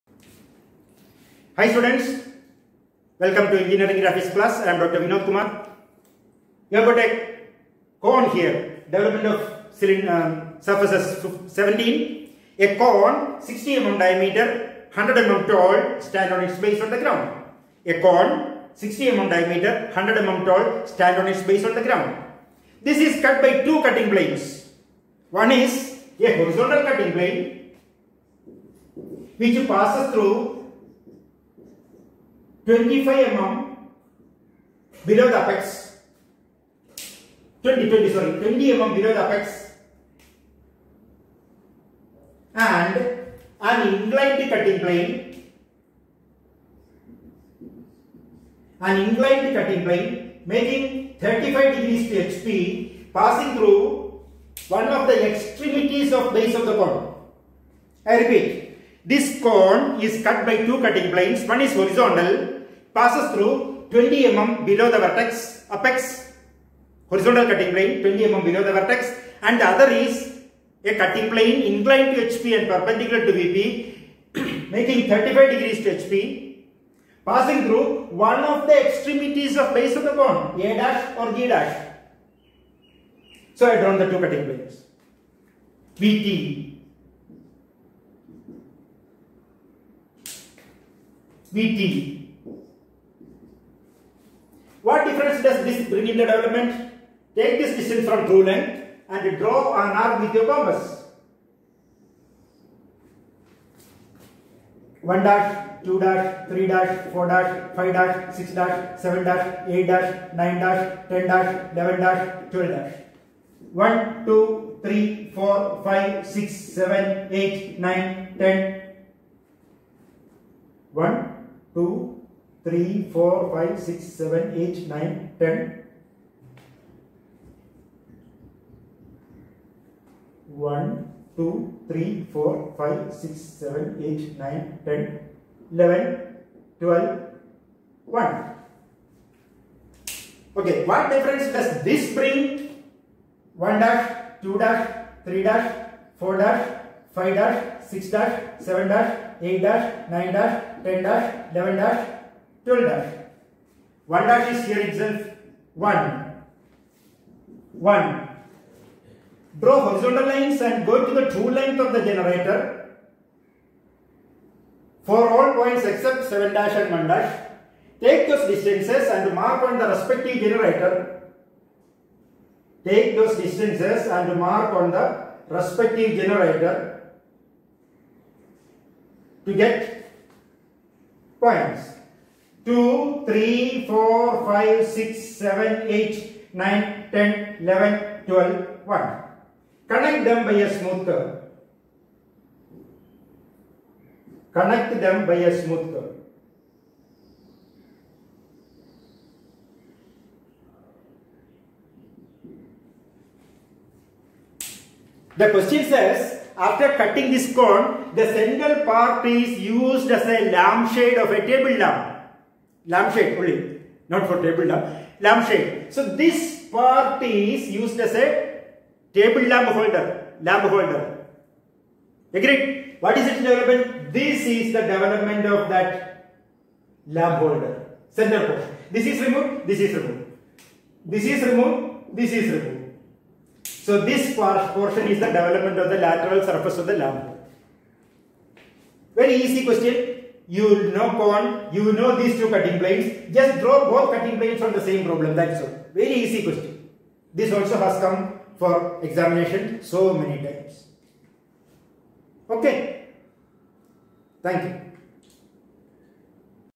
DEVELOPMENT OF SURFACES -17, Solved University Question with two cutting planes by Prof. Vinodkumar
A lamp shade is made by cutting a cone of 60 mm diameter and 100 mm axis by two cutting planes, one parallel to the base and 20 mm below the apex and the other making 35° to the base and passing through one of the extremities of the base of the cone. Draw the development of the lamp shade.